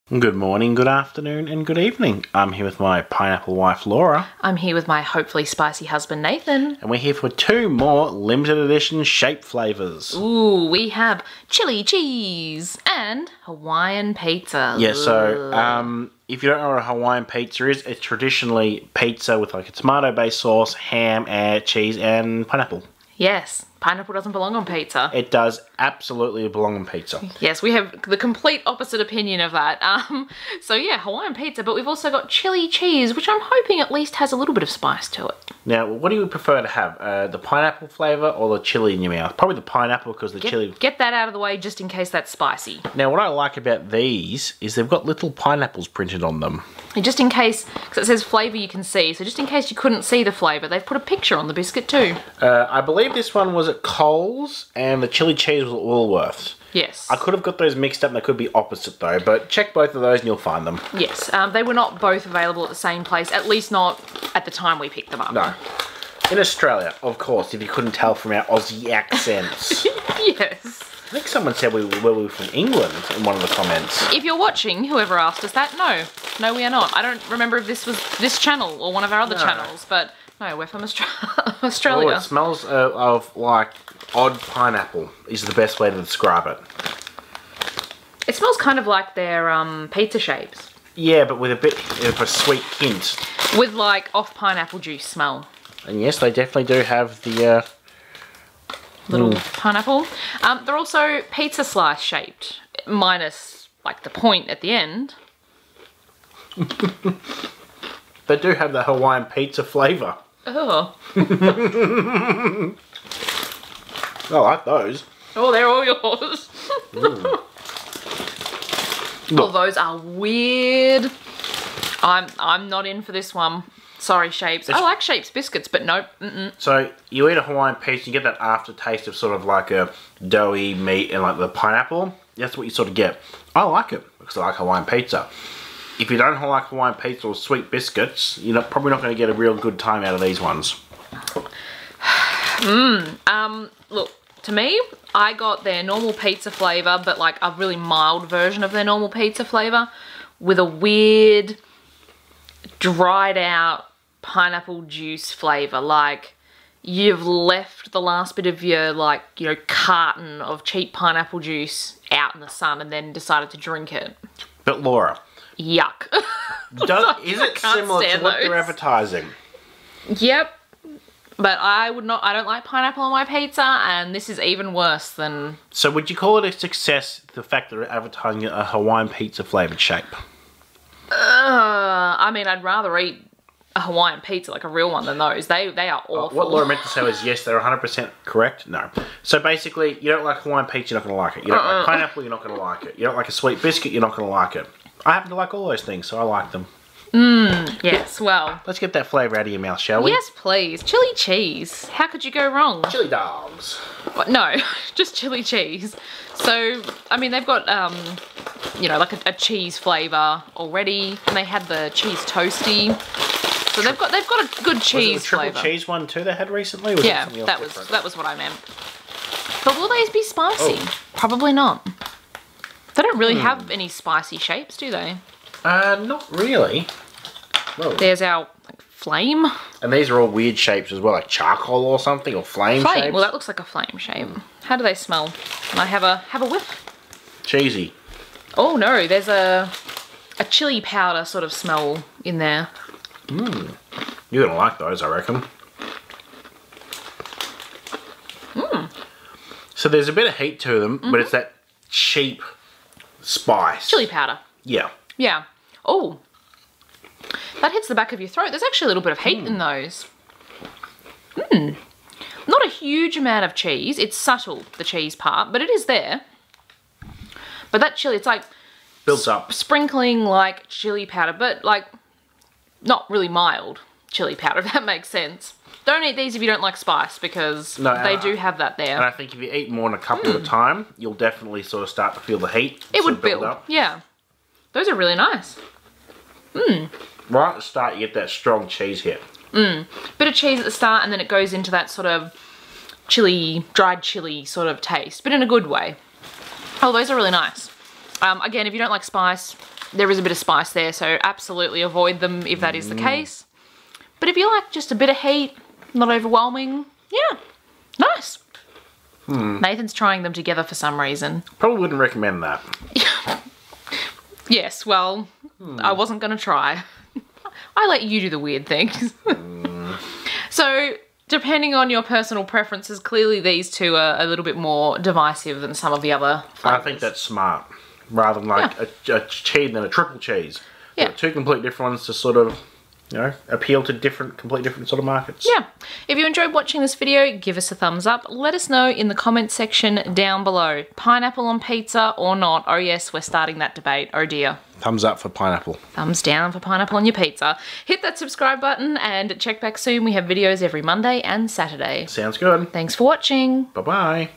Limited Edition Hawaiian Pizza & Chilli Cheese Arnott's Shapes Biscuits Food Review | Birdew Reviews
We taste test and review both of the new Limited Edition Arnott's Shapes Flavours: Hawaiian Pizza and Chilli Cheese!